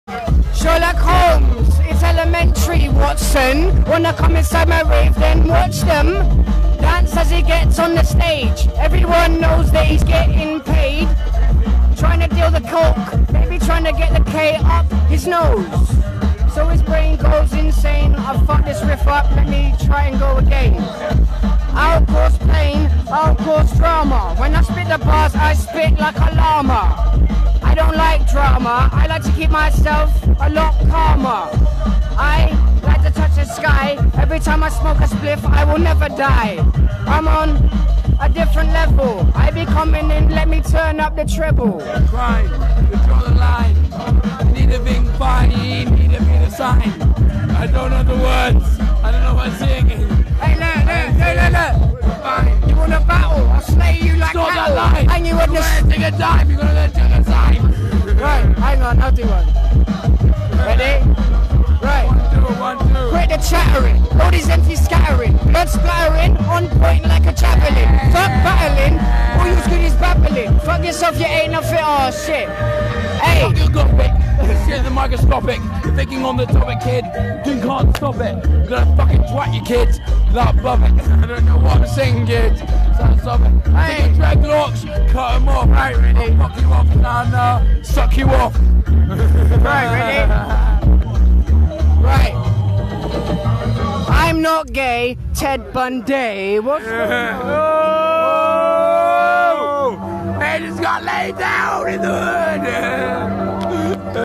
FUNNY 23/5/09 May Bank Holiday Free Party B2B Junglist MC Soundwave VS MC WATSON PT 5
MC Watson VS MC Soundwave Backlash Element Link up highest point in somerset free party